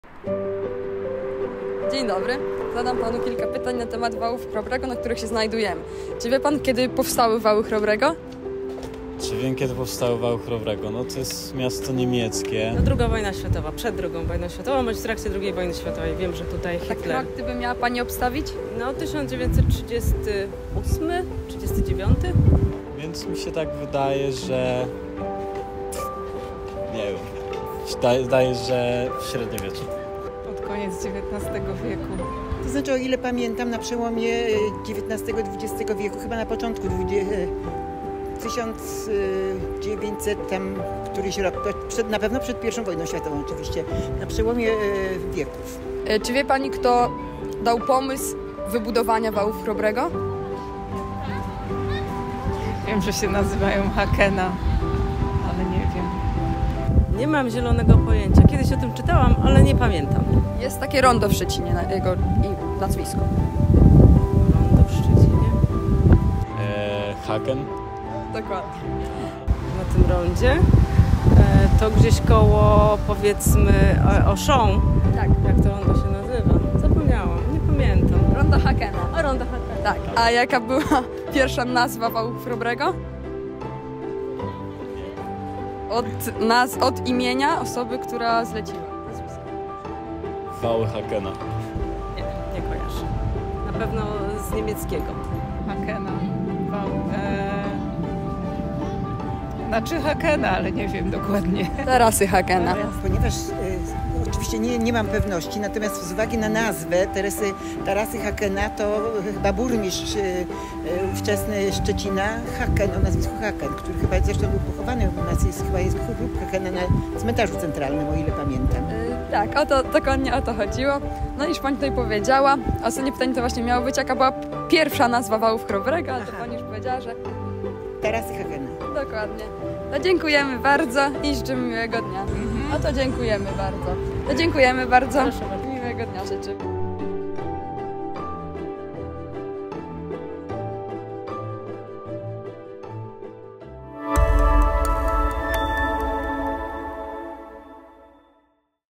Ile mieszkańcy Szczecina wiedzą o Wałach Chrobrego?
Wały Chrobrego, czy też Tarasy Hakena, powstały... w średniowieczu? Sprawdzamy, ile wiedzą mieszkańcy Szczecina o tym wyjątkowym miejscu.